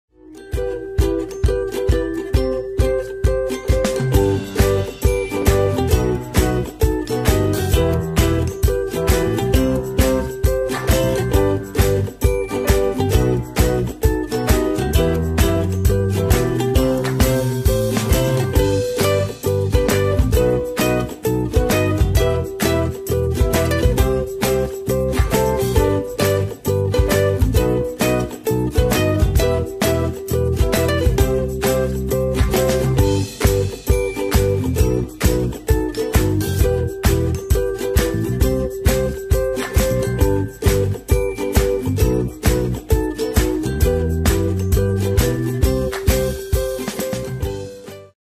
Book Craft
A how to video for making a paper book craft. For different types of worksheets in this style, please